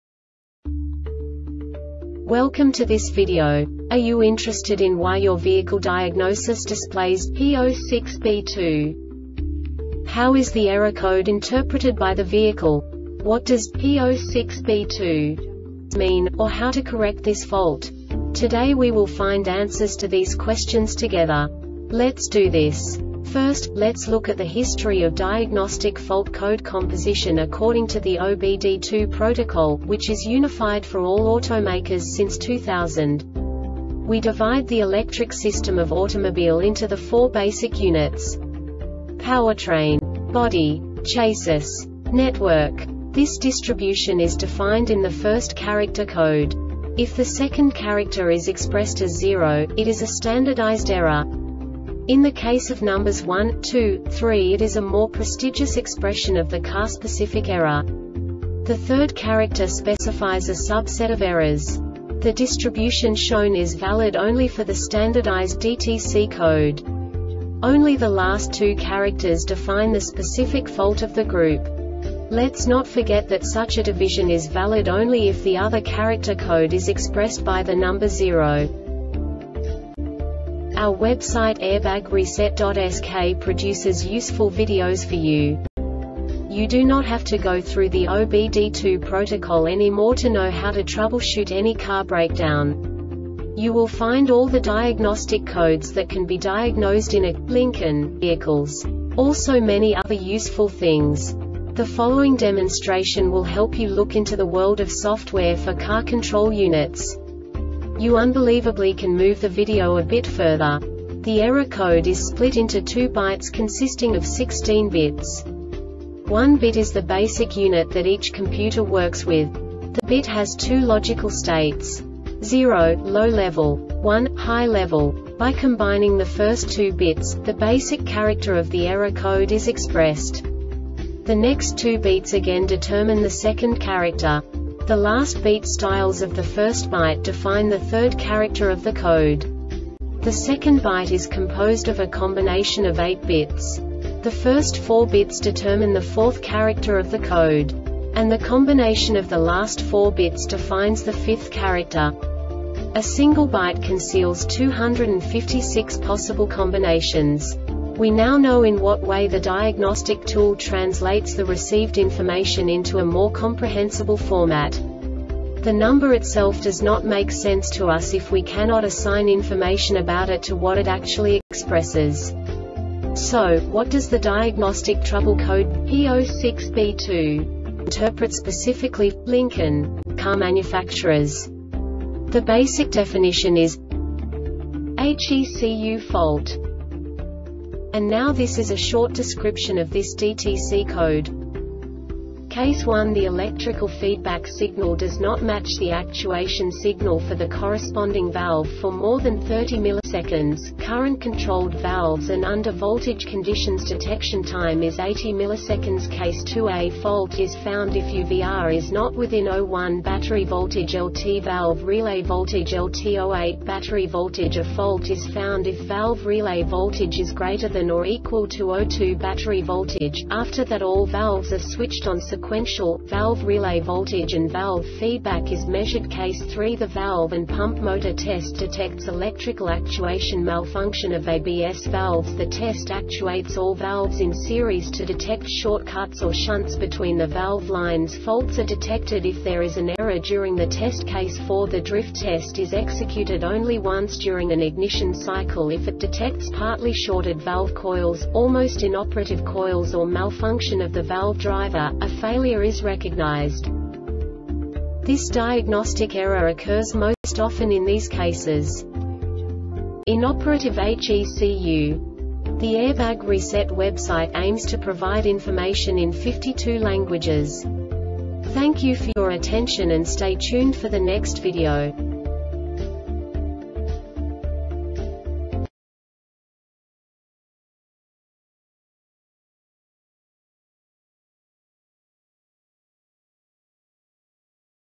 DTC Lincoln P06B2 Short Explanation
Contents:
0:21 Basic DTC analysis according to OBD2 protocol standard.
1:48 Insight into programming
2:58 A diagnostically understandable description of the Diagnostic Trouble Code
3:05 Basic error definition

Make:
Lincoln
Code:
P06B2
Definition:
HECU Fault
Description:
Case 1:The electrical feedback signal does not match the actuation signal for the corresponding valve for more than 30 ms.- Current controlled valves and under voltage conditions :detection time is 80 ms Case 2A Fault is found if UVR is not within 0.1battery voltage valve relay voltage 0.8battery voltageA Fault is found if valve relay voltage ≥ 0.2battery voltage.- After that all valves are switched on sequential,valve relay voltage and valve feedback is measured.Case 3The valve and pump motor test detects electrical actuation malfunction of ABS valves.The test actuates all valves in series (to detect short cuts or shunts between the valve lines).Faults are detected if there is an error during the test.Case 4The drift test is executed only once during an ignition Cycle. If it detects partly shorted valve coils, almost inoperative coils or malfunction of the valve driver,a failure is recognized.
Cause:
 Inoperative HECU